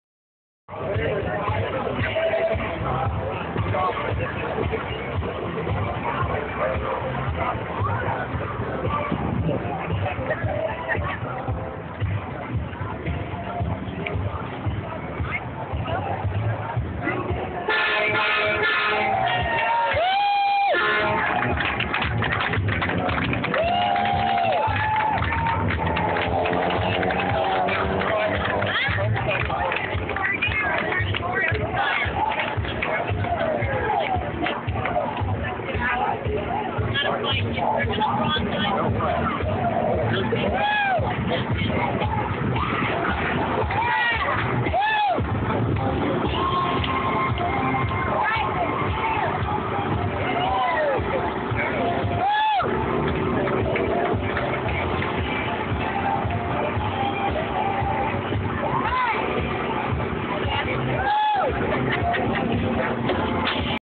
Tea Party Express comes to Gainesville Florida
What an Awesome Day of Fun in the Sun!! Big thanks to the Gainesville Tea Party for pulling this off on very short notice. Great people, inspirational messages and such a Positive Event!!